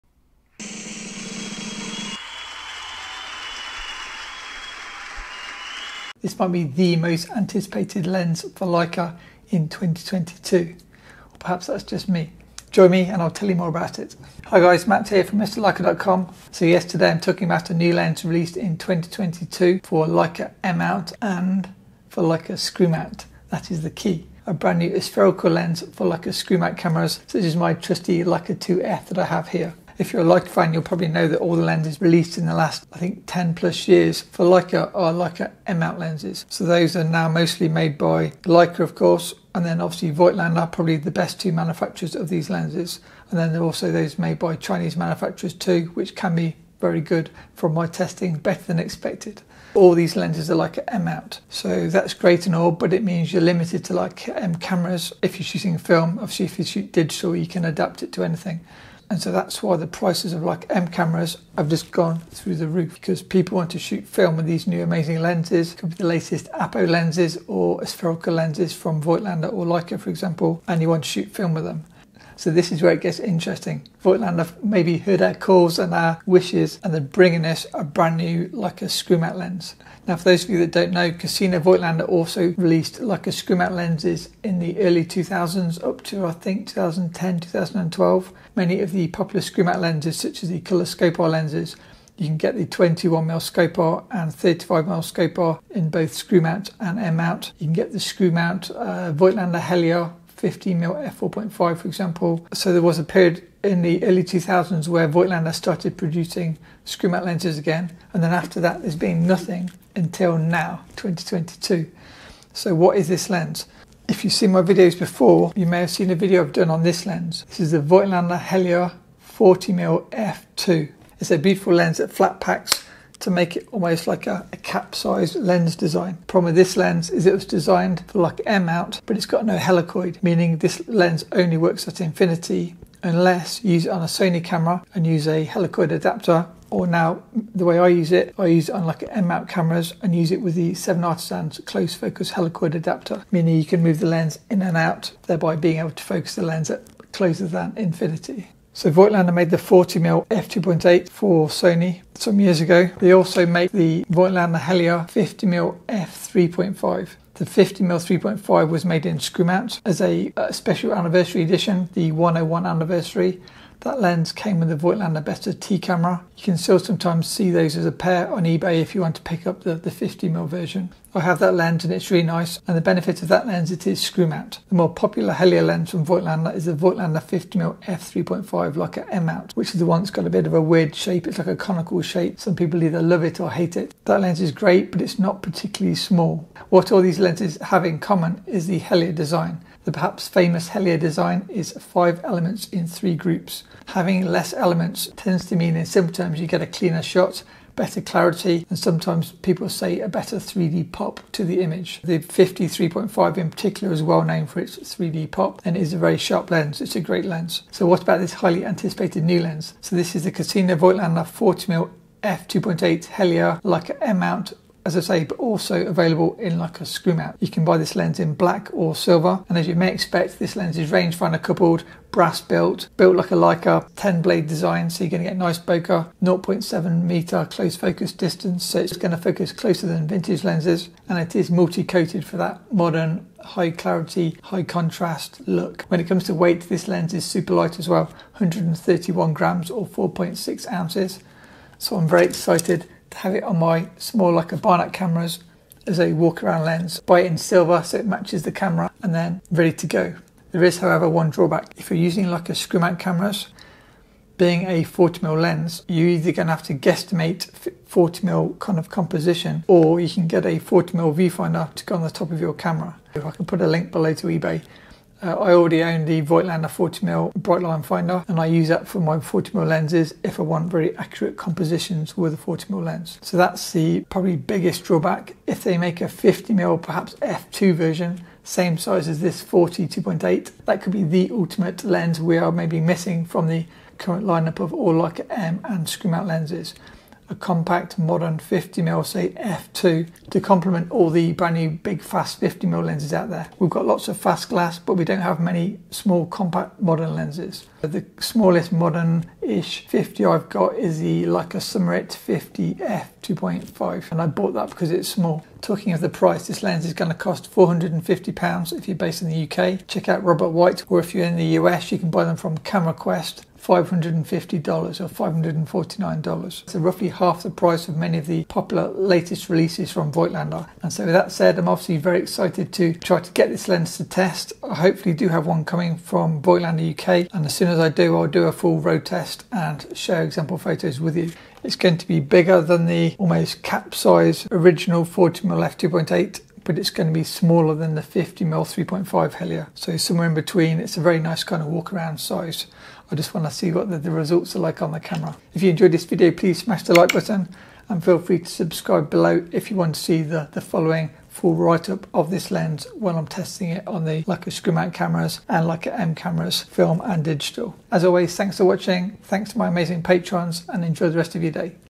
🔴 First New LTM Lens This CENTURY! Voigtlander Heliar 40mm f2.8 (Pre-Look)
Discussing the new lens release from Voigtlander 2022 - Voigtlander Heliar 40mm 2.8 for Leica
📞 ZOOM: Not sure what lens to buy? Arrange a call today

I talk about the new Voigtlander lens for Leica M and Leica screw mount and discuss how it compares to the existing Voigtlander 40 2.8 lens for Sony (Leica M mount*) and Voigtlander 50mm Heliar lenses, Heliar 50mm f3.5 M and LTM versions.
I don't yet have the lens so this is a pre-look and my excitment around the new lens release*